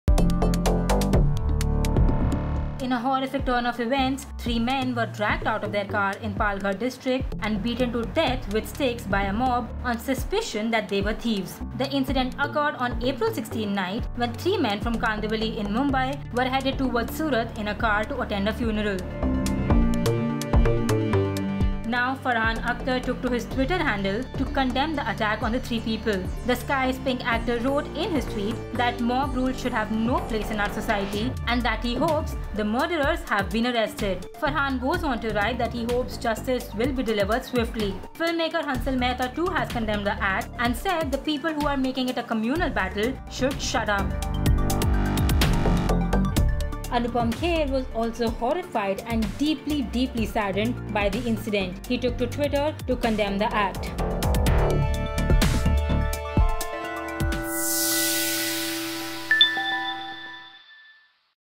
Farhan Akhtar, Anupam Kher and Hansal Mehta CONDEMN Palghar mob lynching
In a horrific turn of events, three men were dragged out of their car in Palghar district and beaten to death with sticks by a mob on suspicion that they were thieves. The Sky Is Pink actor Farhan Akhtar wrote in his tweet that mob rule should have no place in our society and that he hopes the murderers who killed 3 people have been arrested.